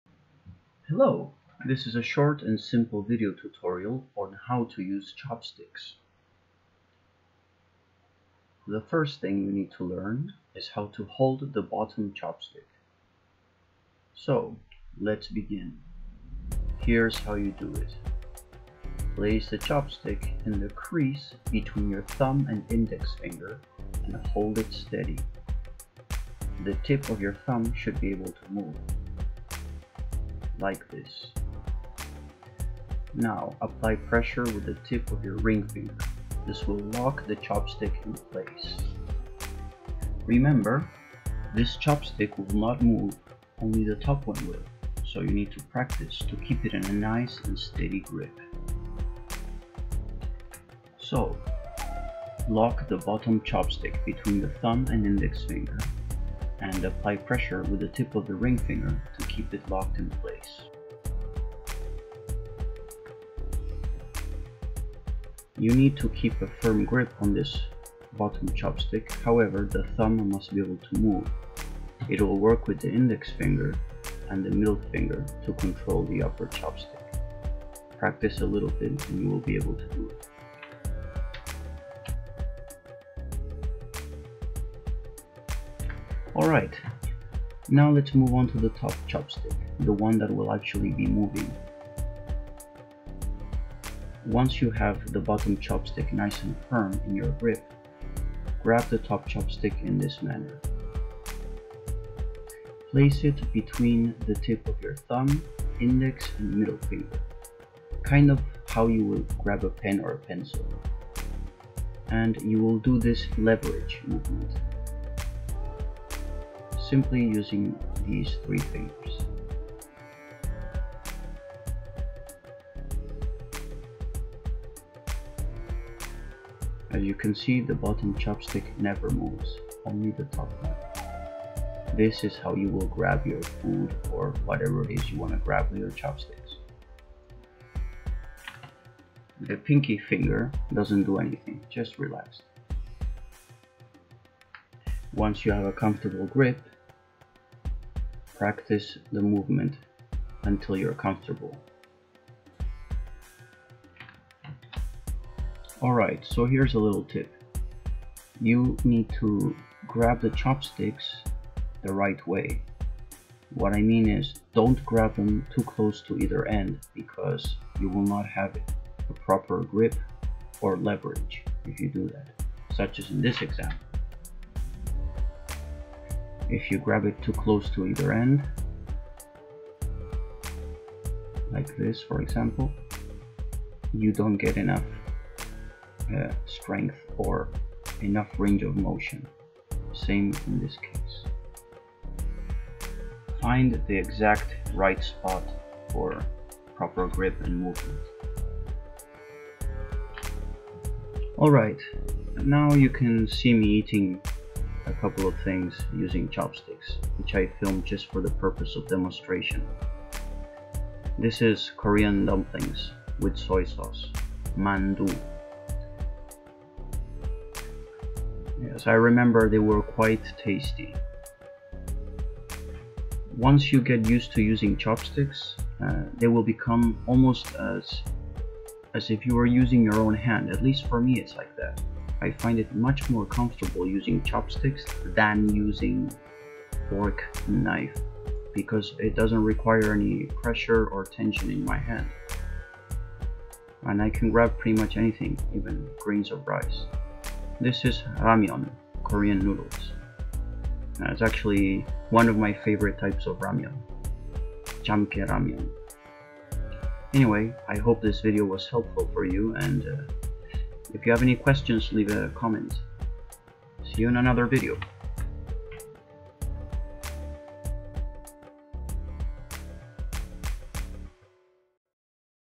How to use chopsticks - Short and easy tutorial
Here's how I use chopsticks. Sorry about the sound quality but I don't have a microphone, I recorded this with my laptop's onboard mic. regardless, I hope you can understand everything I am saying.